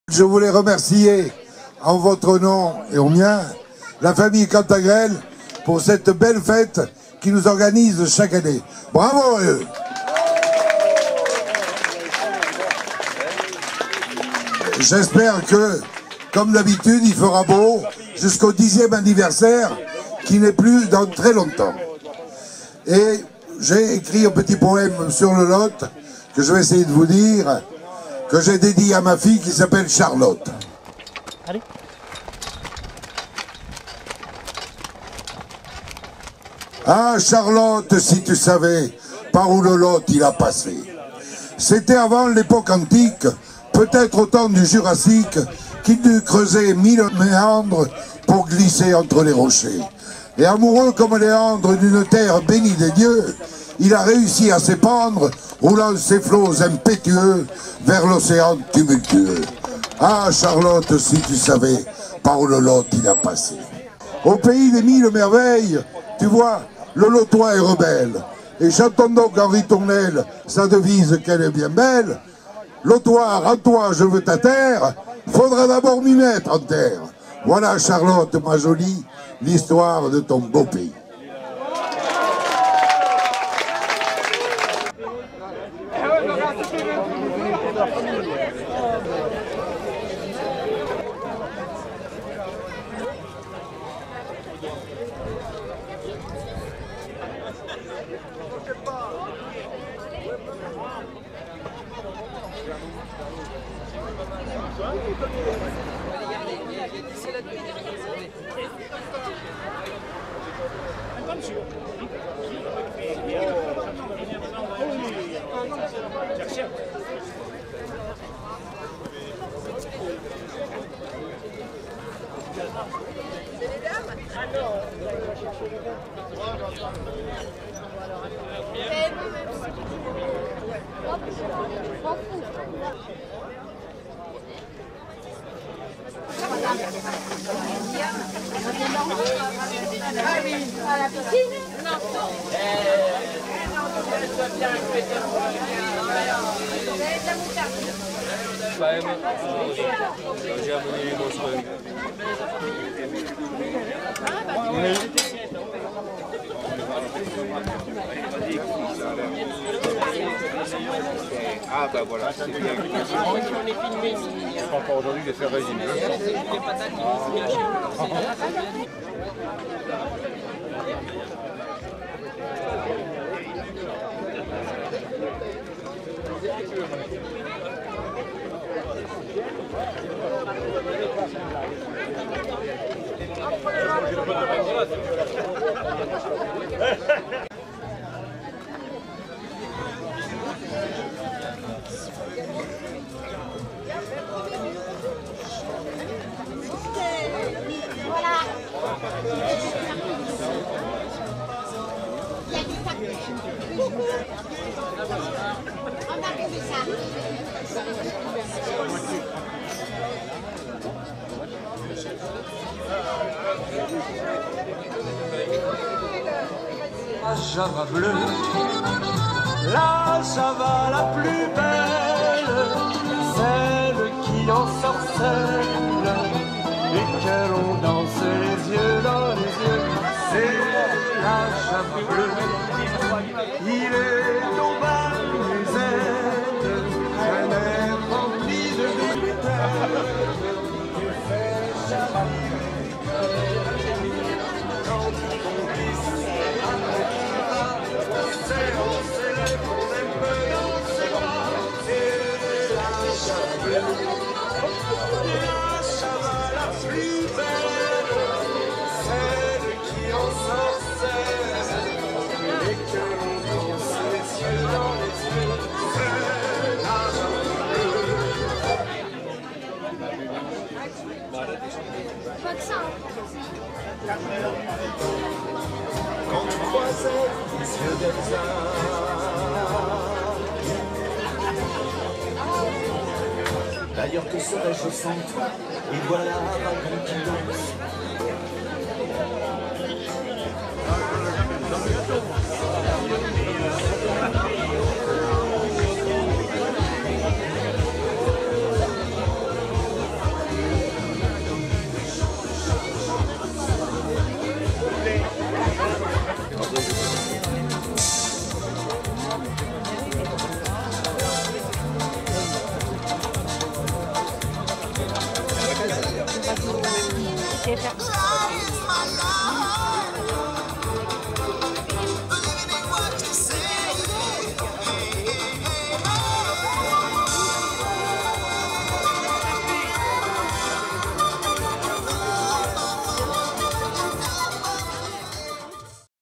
Puy l'Evêque 46 Marché fermier 2017
Une belle journée sur la propriété de la Famille CANTAREL, Viticulteurs à PUY l' EVÊQUE (46)
Appellation CAHORS